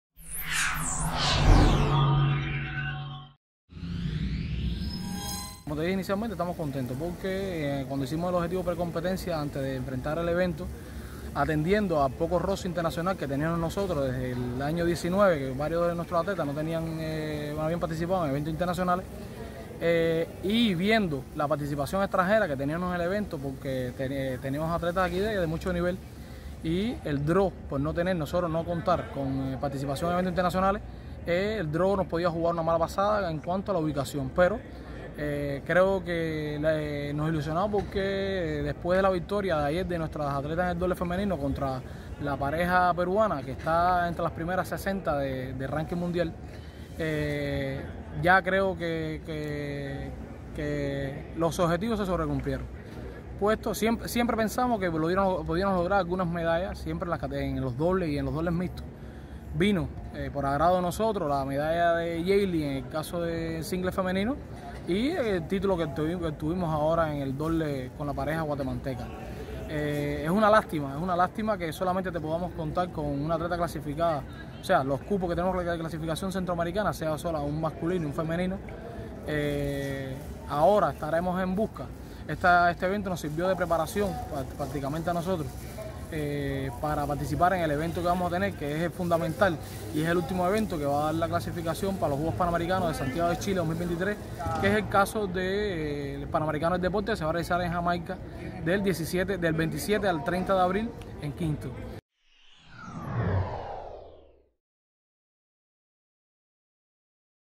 Declaraciones de Orlando Cala Martínez
Orlando Cala Martínez, comisionado nacional de bádminton, hizo declaraciones a JIT sobre actuación cubana en el Giraldilla de La Habana.